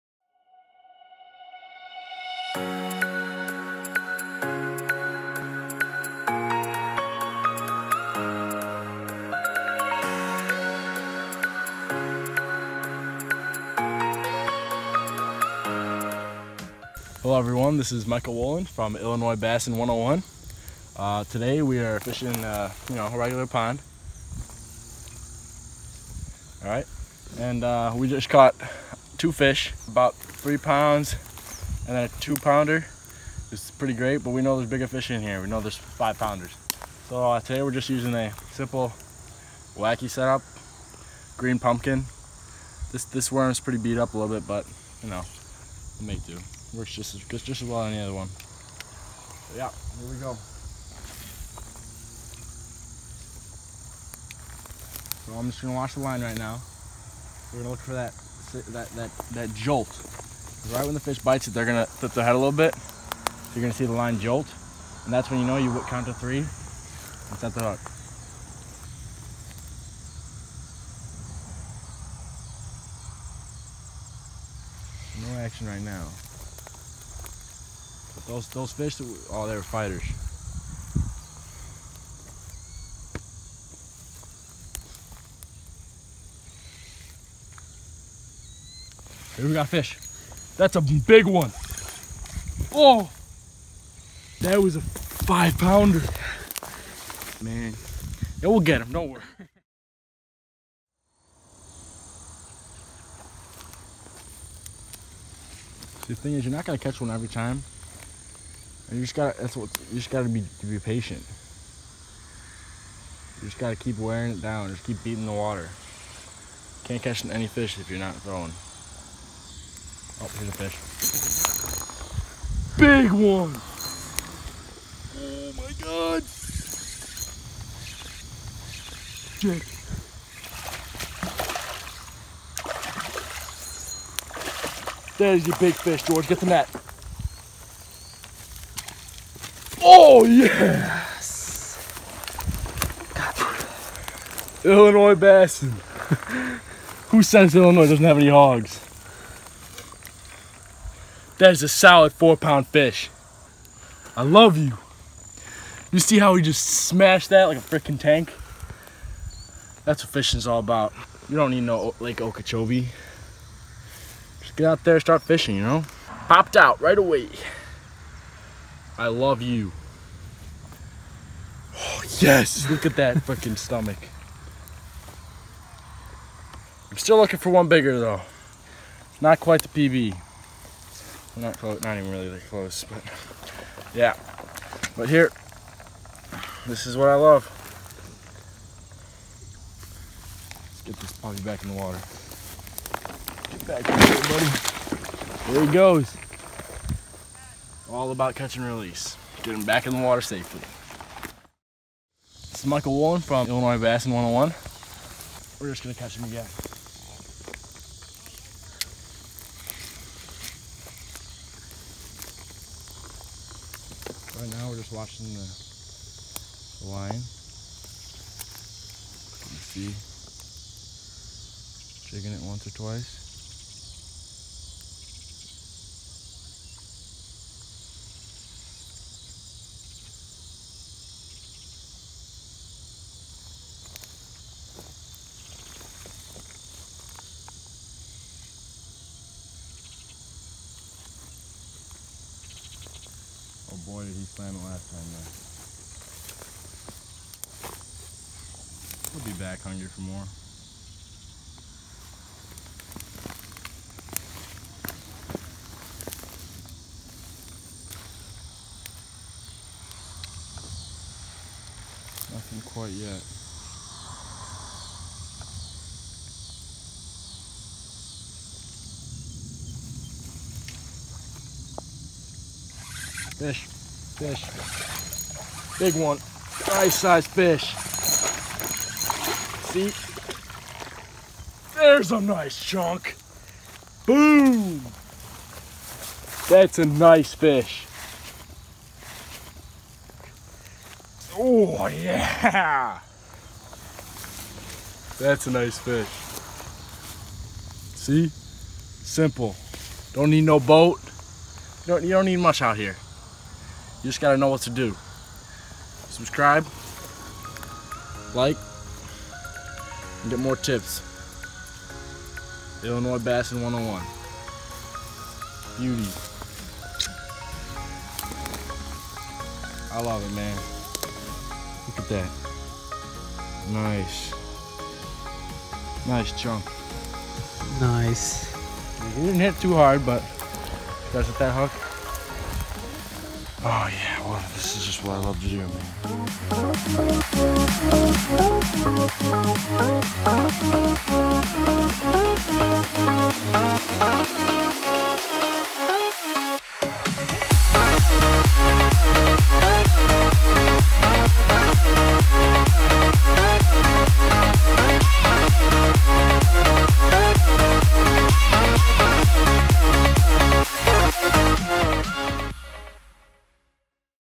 Huge Large mouth Bass caught in Local Illinois Pond: Illinoisbassin101
Hello everyone, this is my second video, this video has way better  bass action and is edited much better. A few very solid largemouth pulled out of one of my local ponds. Until then keep Rippin' Lips